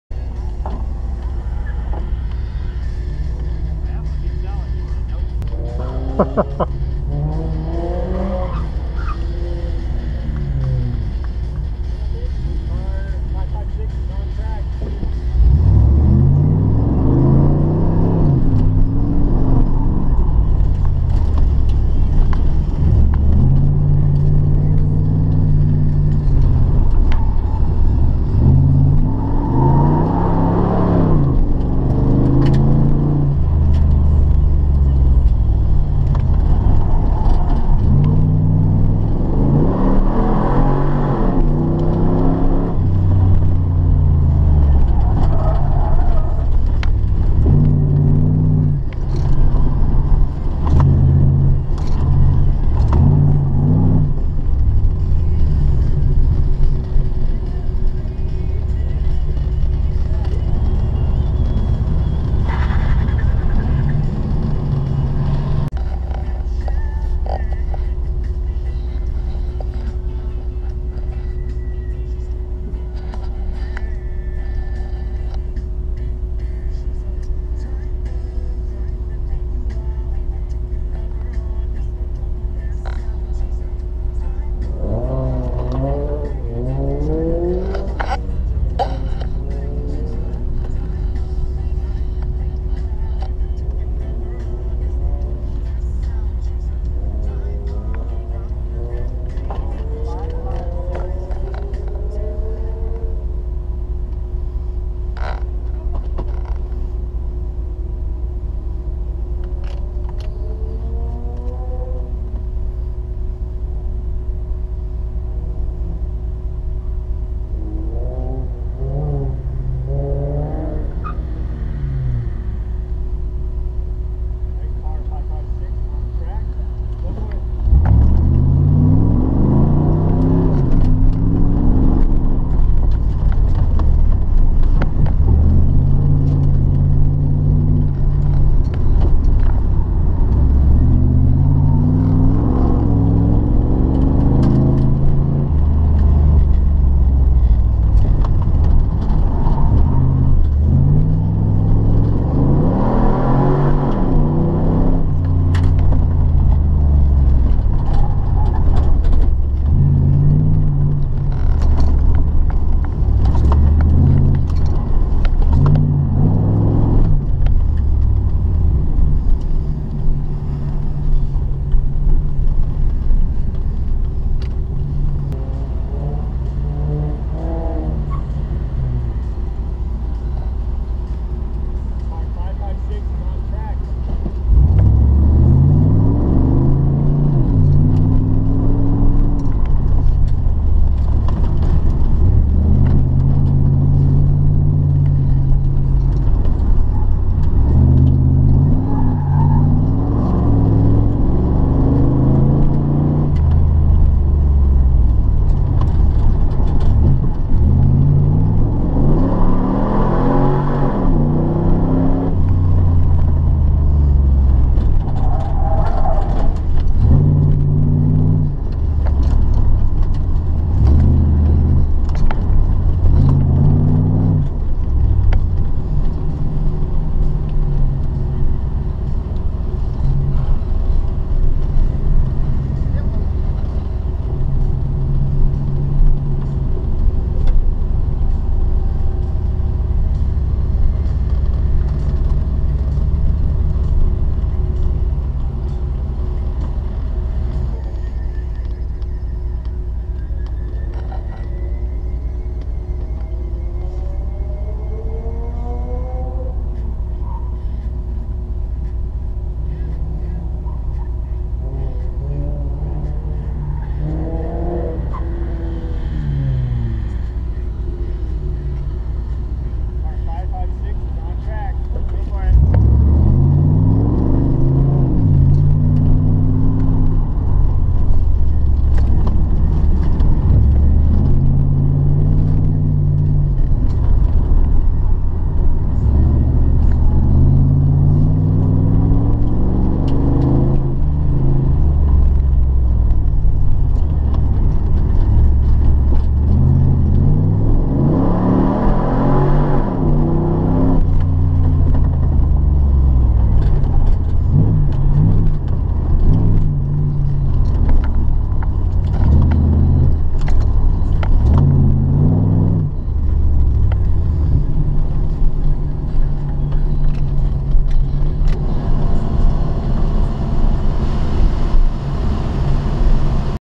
1994 Camaro Z28 Autocross 7/11/21
Arctic Alaska SCCA event #5 out at Alaska Raceway Park.

Just some of my runs from today! Also trying out my new General GMAX RS tires.